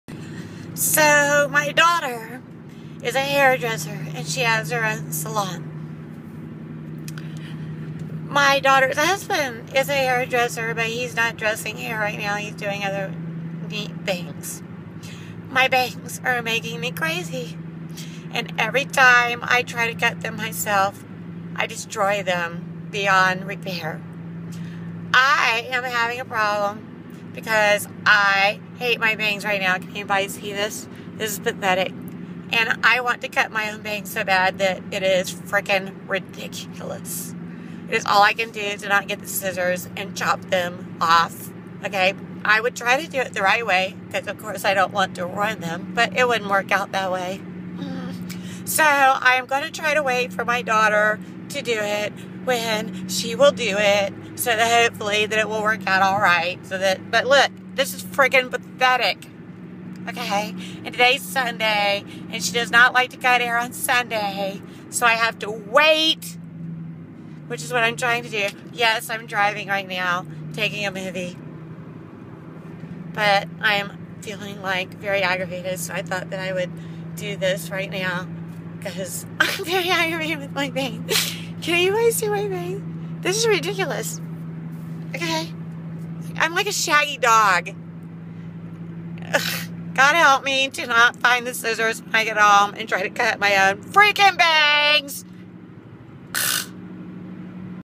Elizabeth White's bangs (Part 1)
My daughter is a master hair dresser, hair extension artist, hair colorist and owns her own salon. She has had years of experience, extensive classes and been to many, many shows. She is a PERFECTIONIST and is precious lol BUT she HATES doing hair on her days off... and Sunday is one of her days off. Turns out that Sunday just so happened to be the day that I ended up having a "Hair Meltdown" with my way too long bangs in my face. So on my way home I took a movie of my bangs and sent it to her lol. Yeah. Because there have been several times in the last few years that I have not been able to wait long enough and tried to trim my own bangs and ended up with the most screwed up bangs EVER. And it took them months to grow out.